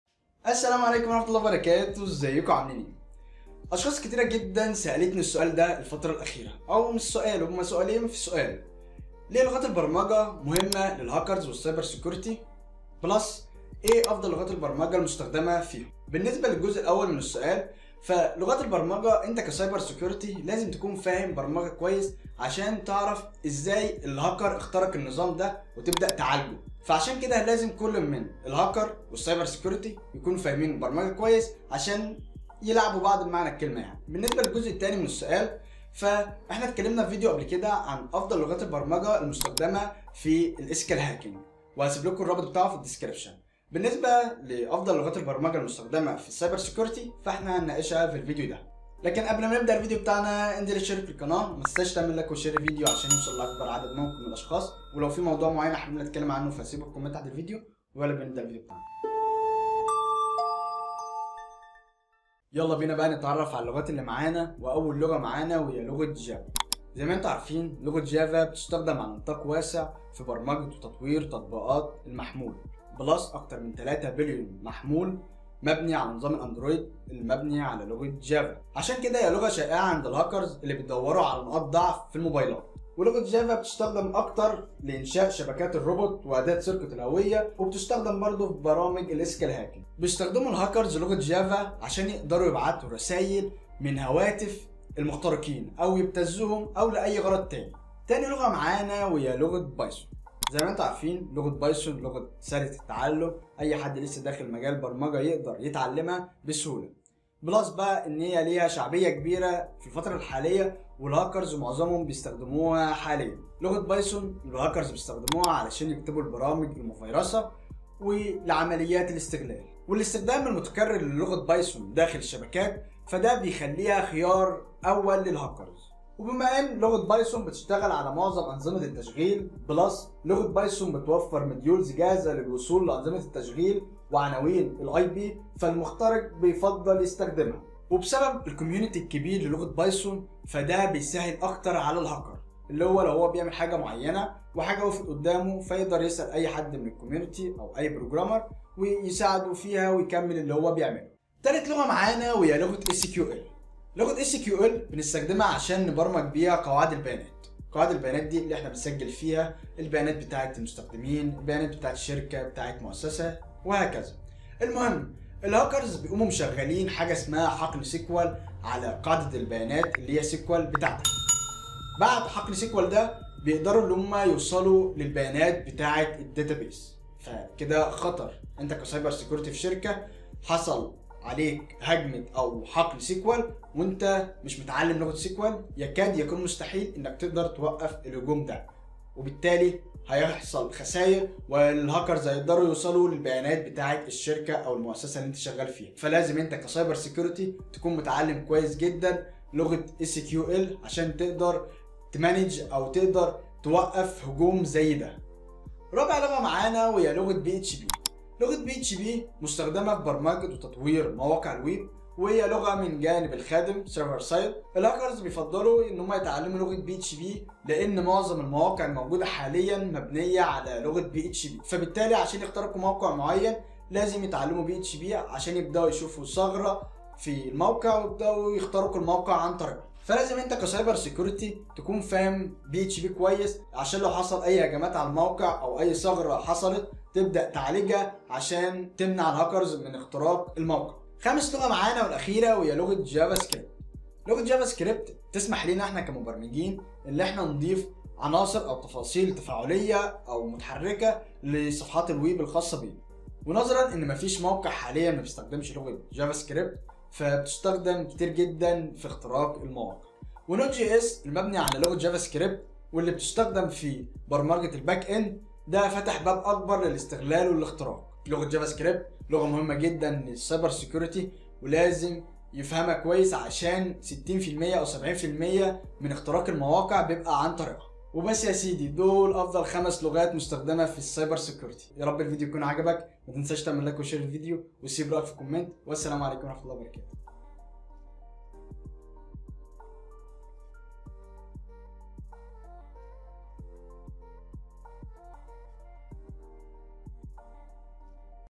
أفضل 5 لغات برمجة مٌستخدمة فى السيبر سيكيورتى | Top 5 programming languages ​​in Cyber ​​Security
سأشارك معكم فى هذا الفيديو أفضل 5 لغات برمجة مٌستخدمة فى السيبر سيكيورتى | Top 5 programming languages ​​in Cyber ​​Security.